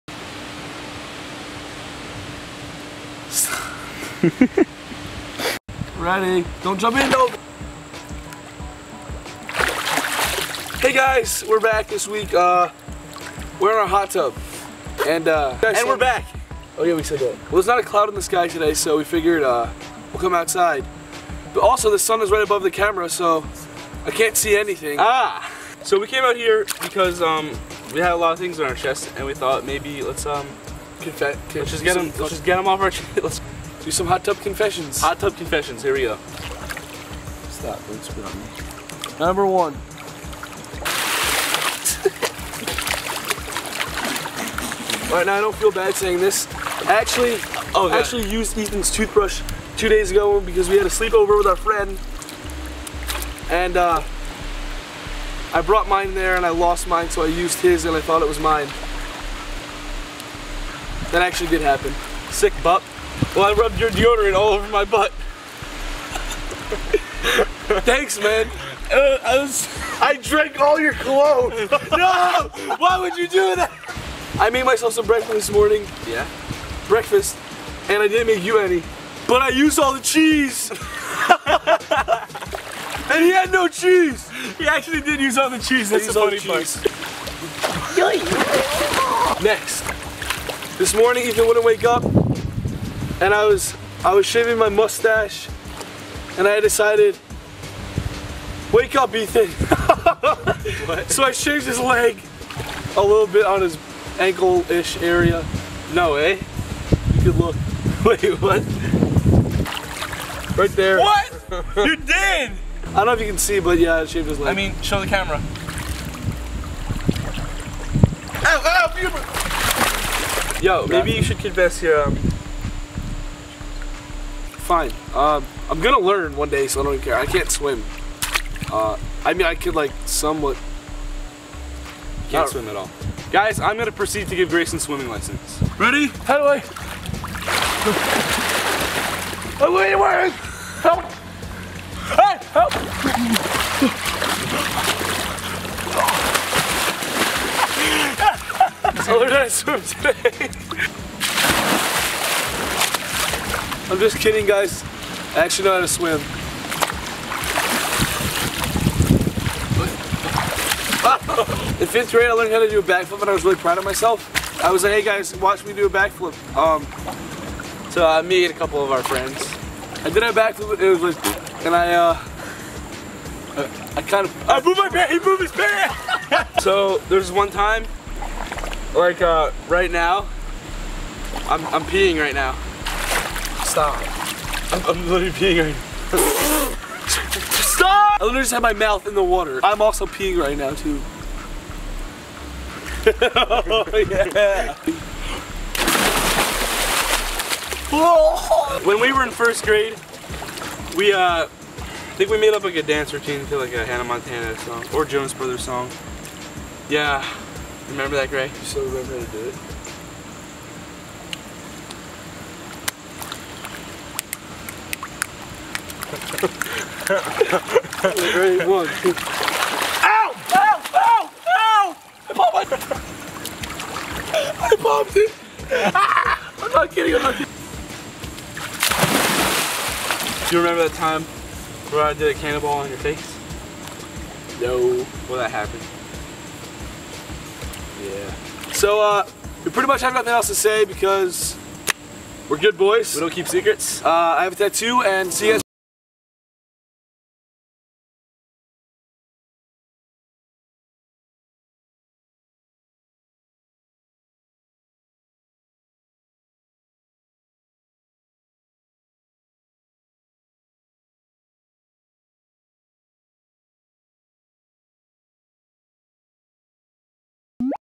Hot Tub Confessions
We tell you guys some never before heard info and stories about the bad and embarrassing things we've done.